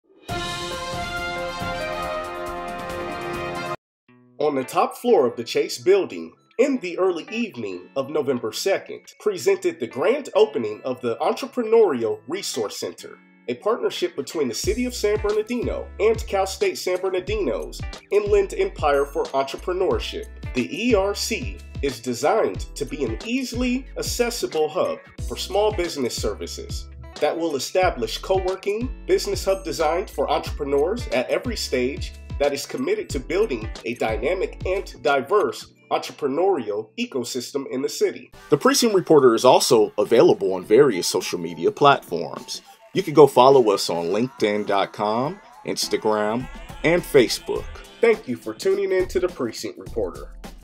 ENTREPRENEURIAL RESOURCE CENTER OPENS IN SAN BERNARDINO
Nearly 100 people filled the Studio D business hub on the top floor of the Chase Building in the early evening of Nov. 2 to officially mark the grand opening of the Entrepreneurial Resource Center (ERC), a partnership between the city of San Bernardino and Cal State San Bernardino’s Inland Empire Center for Entrepreneurship (IECE).

And during the formal portion of the celebration, the words “partnership” and “collaboration” peppered the speeches of the dignitaries in their remarks, for, indeed, the ERC is a place where partnerships can form and collaborative efforts can take place to help aspiring business entrepreneurs launch their dreams and existing small business owners not only thrive, but grow.

“This is a community resource,” said Mike Stull, a professor of entrepreneurship and director of the IECE and the CSUSB School of Entrepreneurship. “We’re, for lack of a better term, we’re an anchor. But we are baked into the project as we’re bringing in other community organizations to be our affiliate partners, to bring in resources and services that either we don’t always provide, or that they’re filling a niche, or they have great relationships in the community that can be leveraged and grown so that we can impact more small businesses. So it’s not just us, it’s a team effort.”

The ERC is designed to be an easily accessible hub for small business services, centrally located in downtown San Bernardino at Studio D, 330 N. D Street. Studio D is a newly established co-working and business hub designed for entrepreneurs at every stage that is committed to building a dynamic and diverse entrepreneurial ecosystem in the city. The ERC will bring together a wide range of entrepreneurial support programs and organizations to provide a complete set of training, business counseling, and access to resources for small business owners and entrepreneurs in the city.

Each partner is making a significant commitment to ensure the success of the ERC, including the city of San Bernardino investing of $1.87 million over three years, and the IECE investing $1.9 million of resources through its major business assistance programs including the Inland Empire Small Business Development Center (IESBDC) and Inland Empire Women’s Business Center (IEWBC).

The ERC grew out of the city and the university’s participation in the Aspen Institute Latinos & Society Program’s City Action Lab, a unique initiative that focuses on Latino majority cities and communities to strengthen local entrepreneurial ecosystems. San Bernardino was one of six cities nationally chosen to participate in the program, which focuses on generating long-term, inclusive economic growth through business ownership.

Out of the city-university partnership with the Aspen Institute, the ERC emerged as one of the ways CSUSB, as an anchor institution in the region, could strategically work with the city.